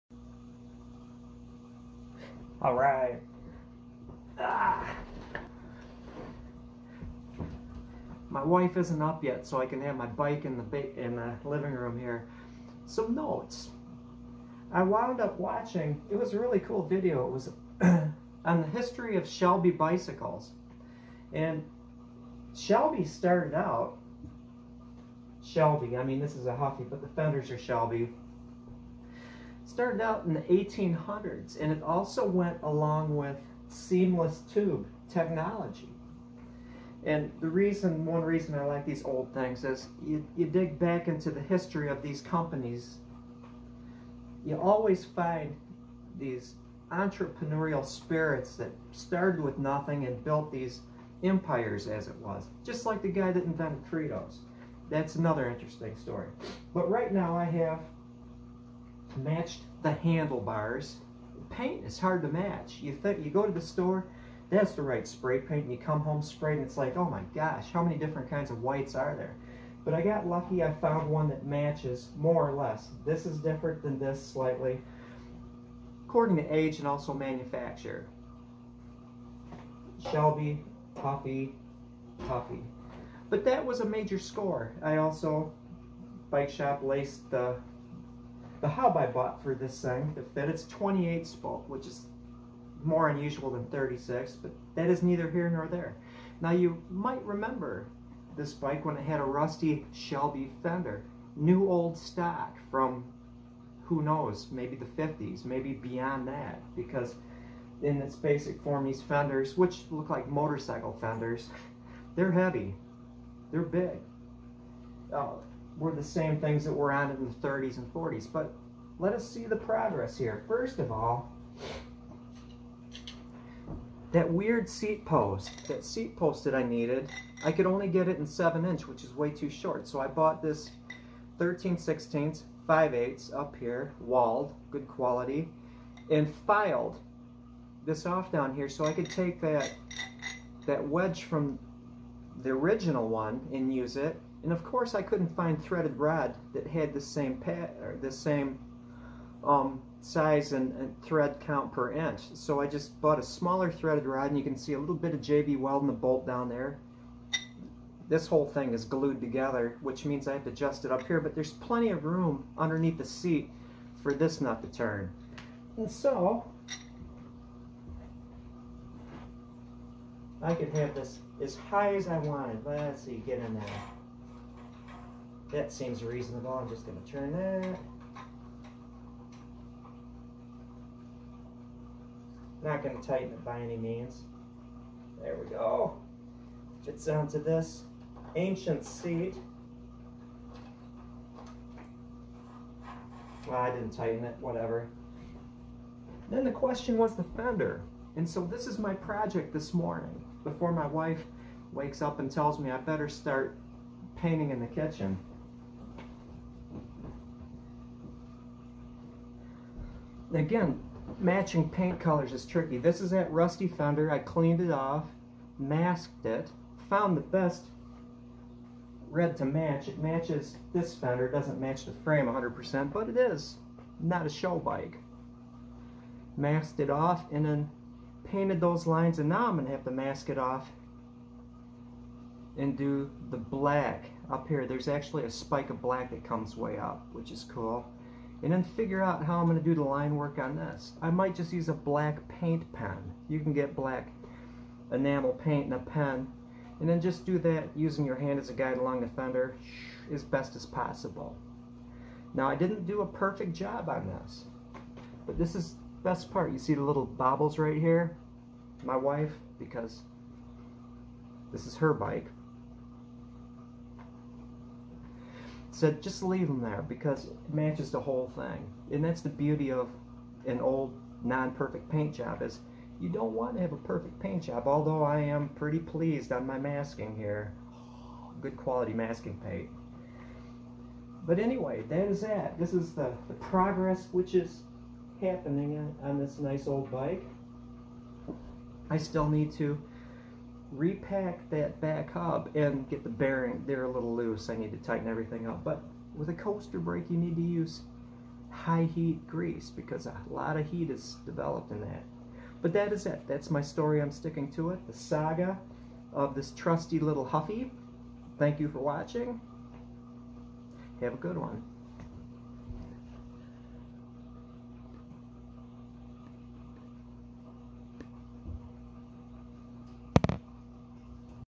Merle Gits'a Fender
I walked the razor's edge and did a reasonable job painting my old Shelby fender. If you're ever bored and have a bit of time, watch the YouTube video, The History of Shelby Bicycles.